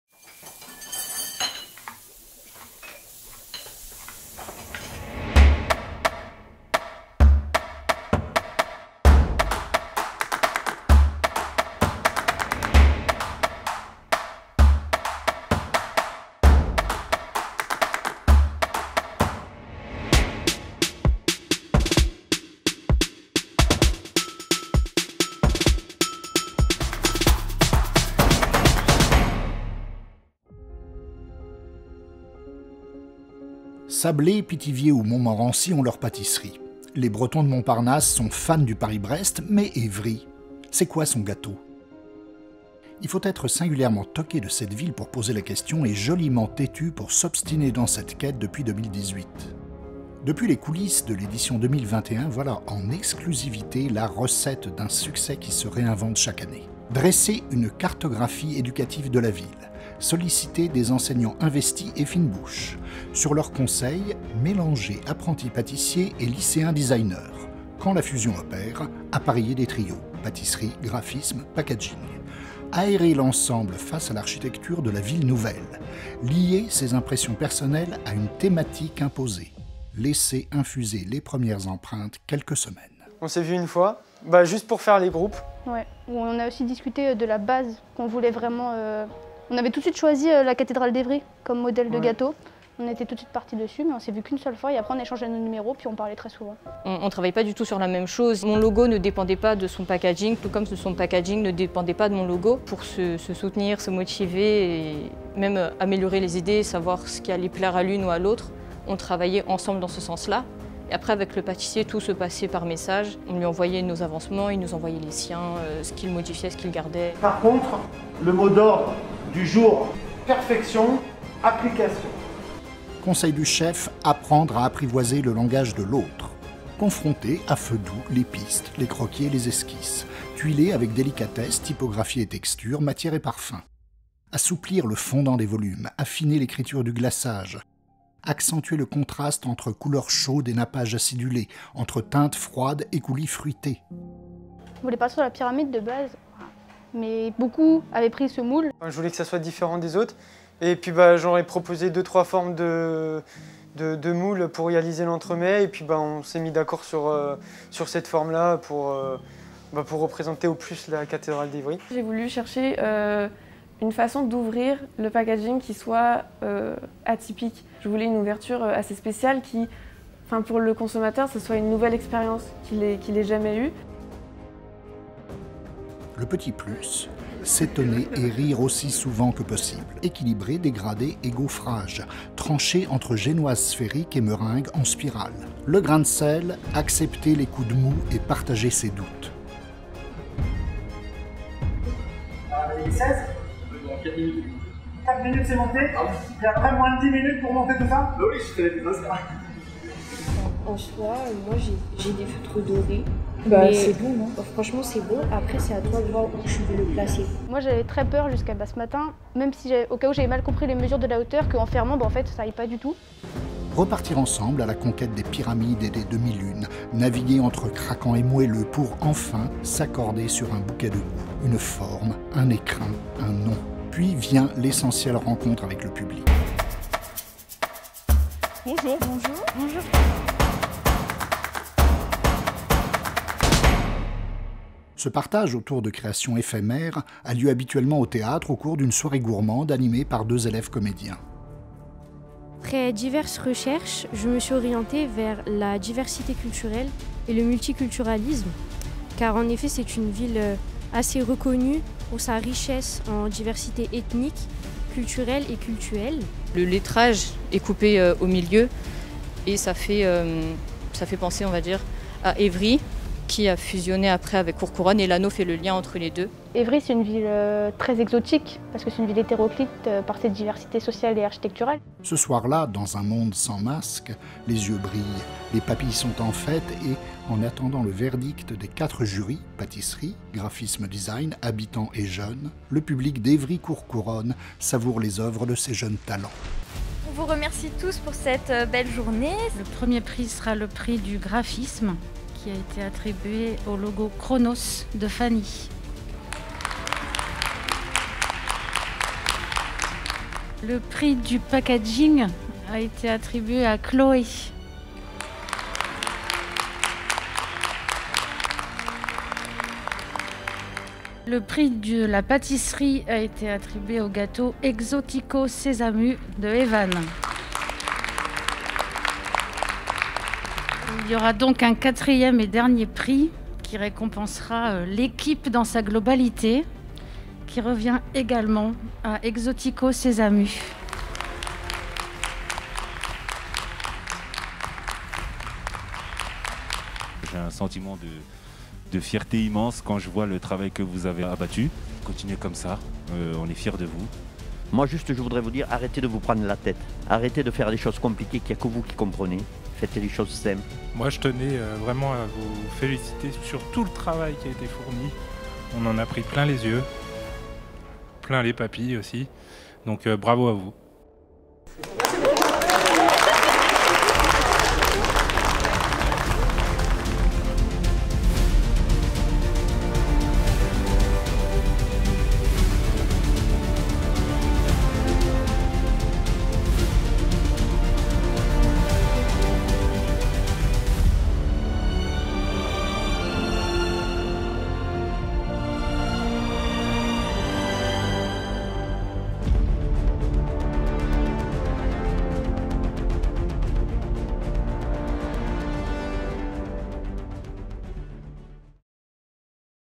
Le Gâteau d'Évry-Courcouronnes 2021 !
Le gâteau d'Évry-Courcouronnes 2021 n'est pas comme les autres... Avec la situation sanitaire actuelle, le concours a été maintenu sans public. Seuls les élèves et le jury de professionnels ont pu participer à cette journée, qui s'est tenue au Théâtre de l'Agora le 4 février. Pour vous plonger dans les coulisses de cette 4ème édition, on vous a concocté un film !

D’après une idée originale de l’Association Préfigurations, en partenariat avec la Faculté des métiers de l’Essonne, le Lycée Georges Brassens d’Évry-Courcouronnes, l’École Départementale de Théâtre-EDT91 et la Scène nationale de l’Essonne.

Membres du jury :
Philippe URRACA – Pâtissier Meilleur Ouvrier de France
Mickaël LA ROSA – Formateur à la FDME
Elsa TOUCHAT - Designer
Sophie BOUCHET - Designer

Apprentis pâtissiers participants de la Faculté des Métiers :
Evan LEBRET, Justine AIOUN, Théo LEFEBVRE, Youma DEMBELE, Emma VIVET, Lucie CADOT, Marie-Thérèse ALFEREZ, Chloé RELAV, Alexane COUTURIER, Mafanta DIAKITE

Élèves participants du Lycée Georges Brassens Designers d’espace :
Margaux AUGENDRE, Soazic BUSSON, Marine PARREIRA SERRE, Solène LOGE, Léa TRILLES CLAUDEL, Doreen LIEGEOIS, Louna DJAE, Anas BELQASMI, Théo PINEAU, Clélie MONGUILLON, Lou CHINIARD, Tessa AIGLE, Chloé SERRE, Rachel GAUTHIER, Fabian MONTES ESCAMEZ, Maroua ZAROUG

Élèves participants du Lycée Georges Brassens Designers graphistes :
Mathilde ANGLEVIEL COIRARD, Loralya LEBON, Fanny GASQ, Victor BONNIN, Hugo FRITZ, Inès RAPIN, Baptiste LHOST, Sullivan BARAIZE, Hugo LUQUET, Maëlle ROULLIS, Célestine FONTAINE, Wahid KERMZI, Bryan AMEGNIDO, Kathleen CASTEL

Prix de la meilleure collab’ gâteau/packaging/design
Evan LEBRET – pâtissier
Margaux AUGENDRE – designer
Mathilde ANGLEVIEL COIRARD – graphiste

Coup de cœur du jury - meilleur gâteau
Evan LEBRET

Coup de cœur du jury - meilleur logo
Fanny GASQ

Coup de cœur du jury - meilleur packaging
Chloé SERRE

Texte et voix : Xavier Brière, Directeur de l'École Départementale de Théâtre EDT-91
Images : Nicolas Jalu, Florian Kuenemann, Fabrice Oussou – COLLECTIF BKE | Montage vidéo : Robin Carpentier – COLLECTIF BKE